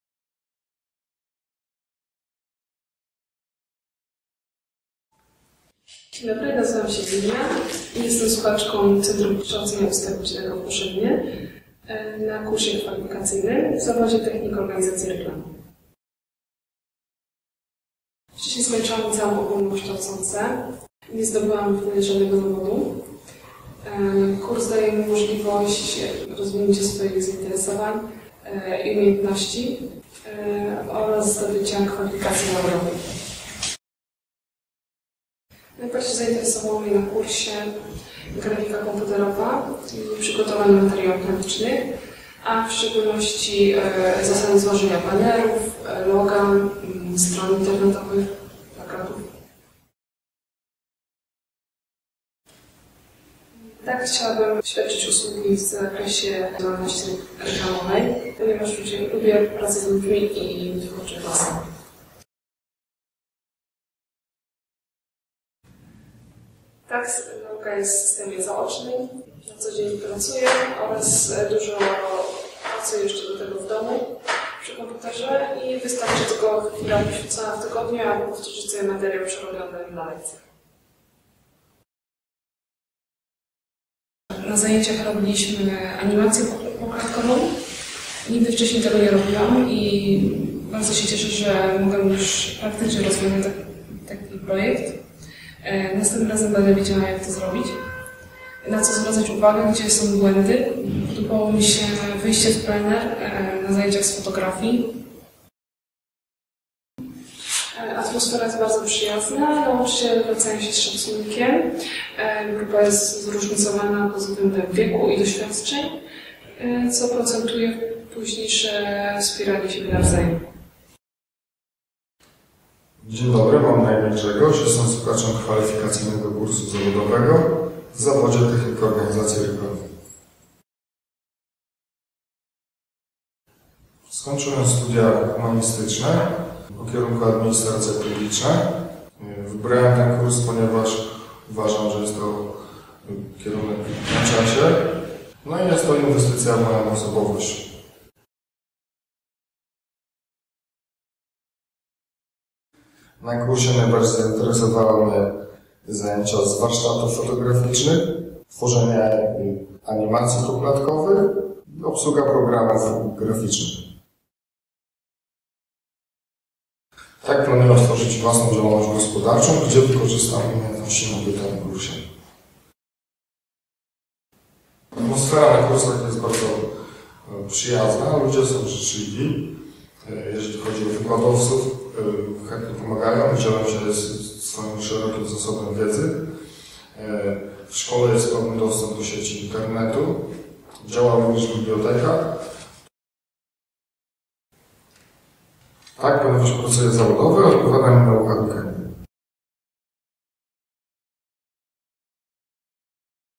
Wywiad ze słuchaczami - technik organizacji reklamy - CKU Koszalin
Wywiad ze słuchaczami kierunku technik organizacji reklamy - CKU Koszalin